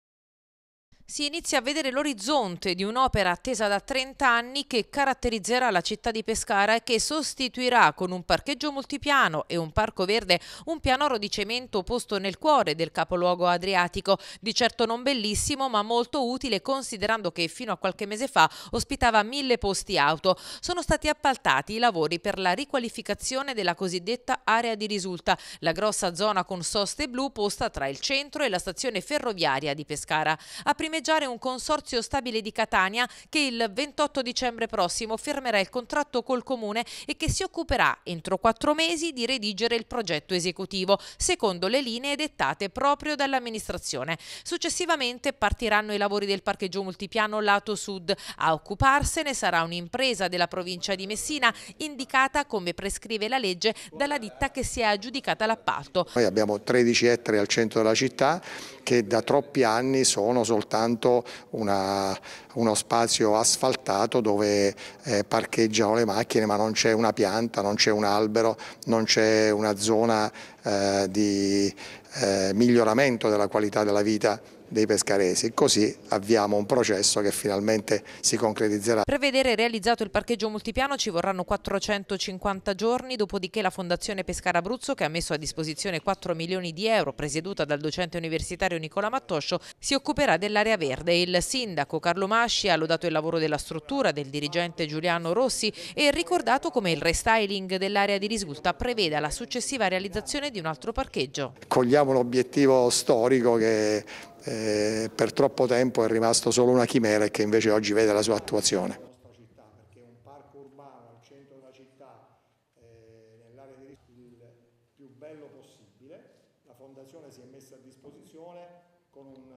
Pescara, restyling area di risulta: entro il 2026 il parcheggio multipiano - 23/11/2024
Servizi TG | Pescara, restyling area di risulta: entro il 2026 il parcheggio multipiano | 23/11/2024